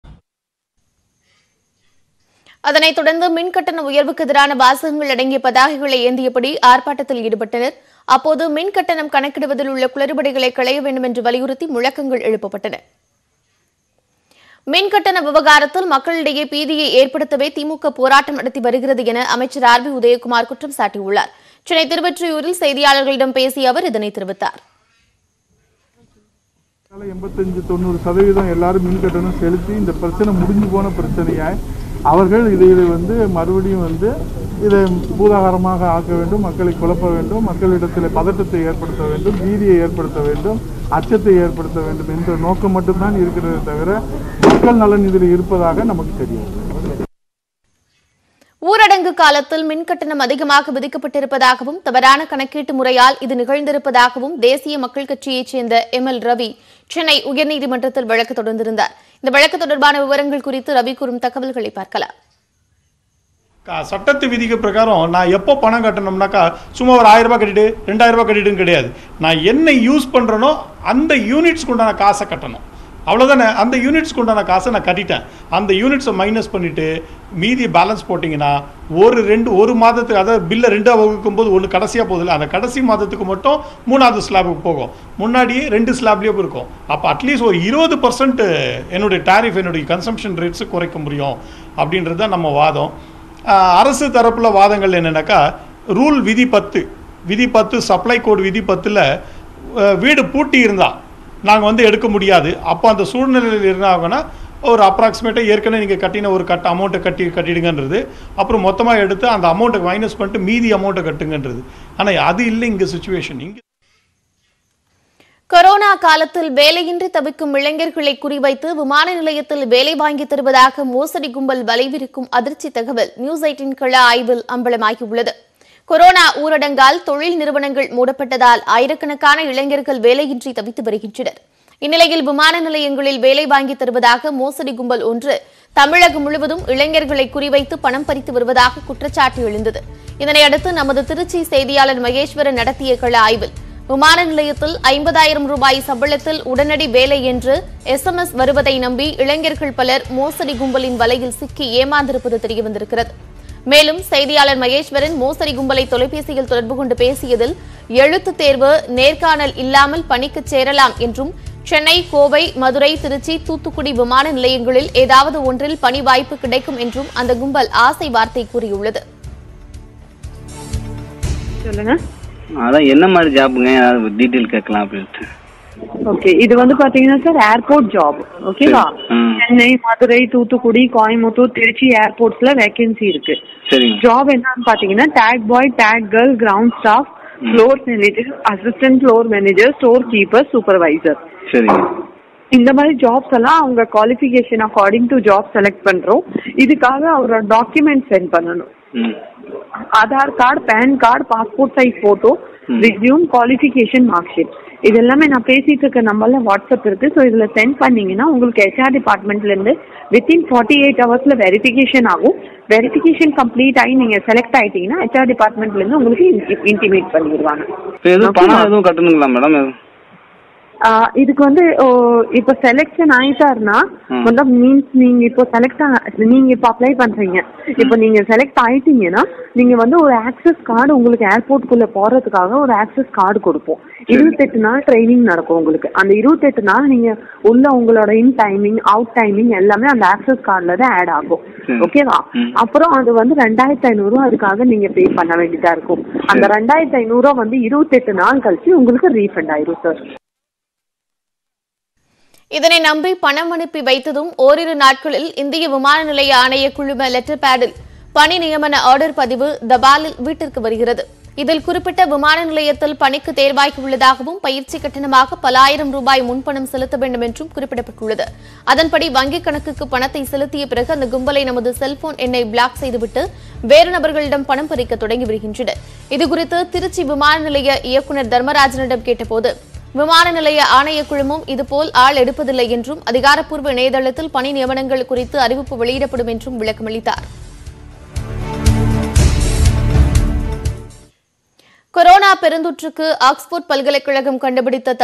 மின் கட்டணம் கணக்கிடுவதில் உள்ள குளறுபடிகளை கலையவேண்டும் என்று வலியுறுத்தி முழக்கம் | Tamil News
தொடரும் மின் கட்டண குழப்பம் | மின் கட்டணம் கணக்கிடுவதில் உள்ள குளறுபடிகளை கலையவேண்டும் என்று வலியுறுத்தி முழக்கங்கள் எழுப்பப்பட்டன | Tamil News